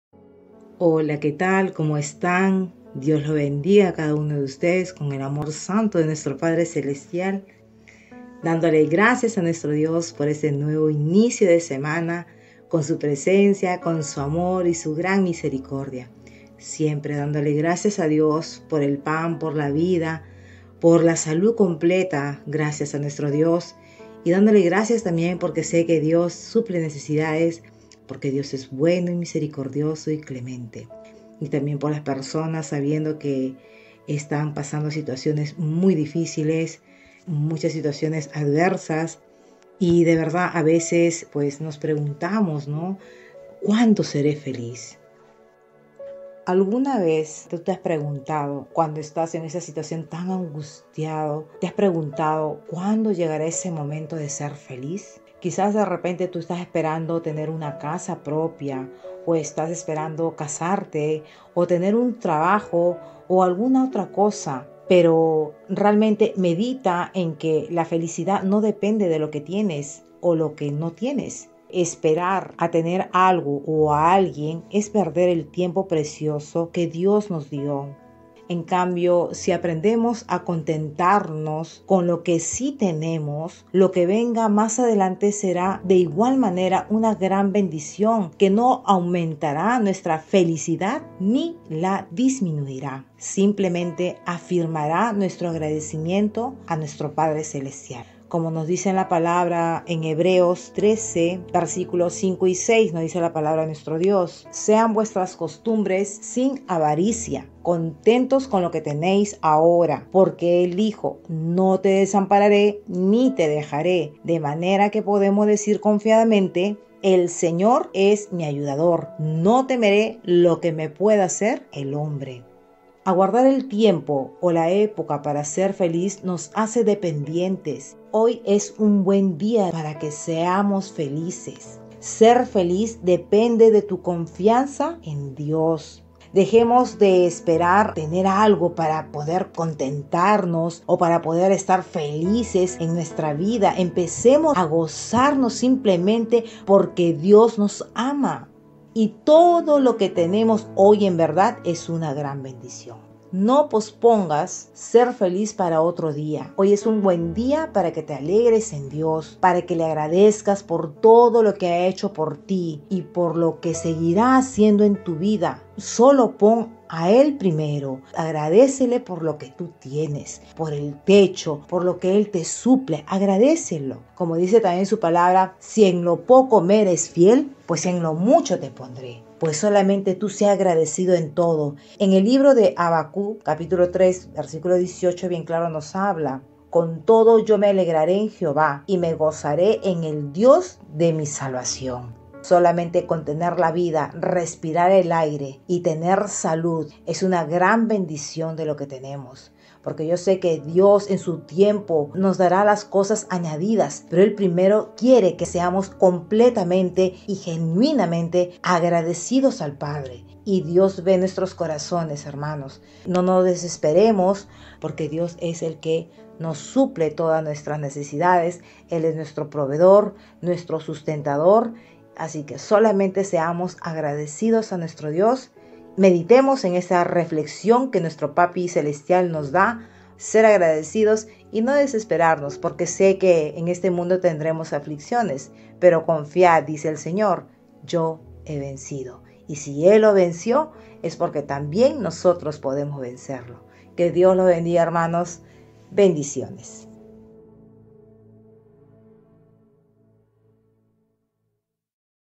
¿Eres feliz?
Reflexión de la semana a cargo de la sierva Eveli Lazo De La Vega

Audio instrumental de fondo obtenido del canal: EU Producciones Musicales